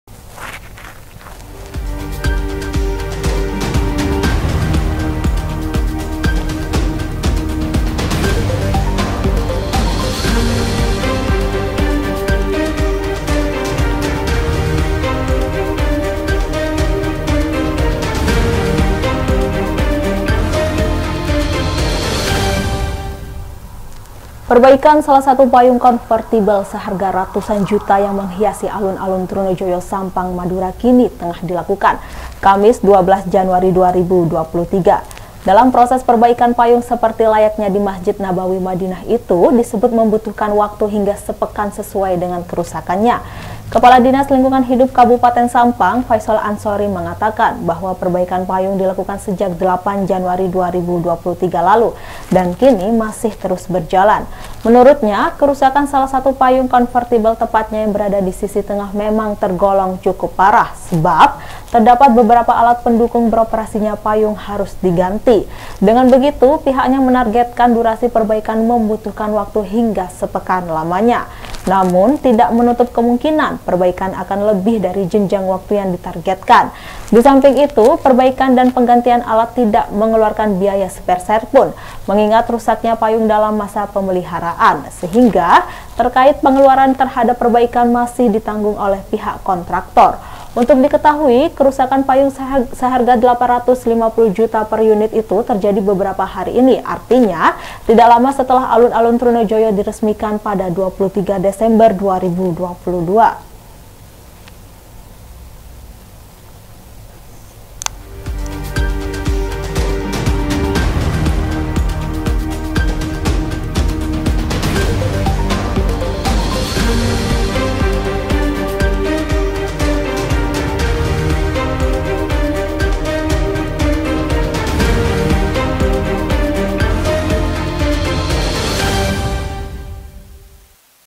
Perbaikan Kerusakan Payung Seharga Ratusan Juta di Sampang Butuh Waktu Sepekan
SAMPANG - Perbaikan salah satu payung konvertibel seharga ratusan juta yang menghiasi Alun-Alun Trunojoyo Sampang, Madura kini tengah dilakukan, Kamis (12/1/2023).

Dalam proses perbaikan payung seperti layaknya di Masjid Nabawi, Madinah itu disebut membutuhkan waktu hingga sepekan sesuai dengan kerusakannya.

Kepala Dinas Lingkungan Hidup (DLH) Sampang Faisol Ansori mengatakan bahwa perbaikan payung dilakukan sejak (8/1/2023) lalu dan kini masih terus berjalan.

Menurutnya, kerusakan salah satu payung konvertibel tepatnya yang berada di sisi tengah memang tergolong cukup parah.

Sebab terdapat beberapa alat pendukung beroperasinya payung harus diganti.

"Termasuk upaya penanggulangan perbaikan secara reset ulang software," ujarnya.

Dengan begitu, pihaknya menargetkan durasi perbaikan membutuhkan waktu hingga sepekan lamanya.

Namun, tidak menutup kemungkinan, perbaikan akan lebih dari jenjang waktu yang ditargetkan.

"Target kami perbaikan rampung satu pekan ini," terangnya.

Di samping itu perbaikan dan penggantian alat tidak mengeluarkan biaya sepeserpun, mengingat rusaknya payung dalam masa pemeliharaan.

Sehingga terkait pengeluaran terhadap perbaikan masih ditanggung oleh pihak kontraktor.

"Kerusakan tidak ditanggung kami," pungkasnya.

Untuk diketahui, kerusakan payung seharga Rp 850 juta per unit itu terjadi beberapa hari ini, artinya tidak lama setelah Alun-Alun Trunojoyo diresmikan pada (23/12/2023).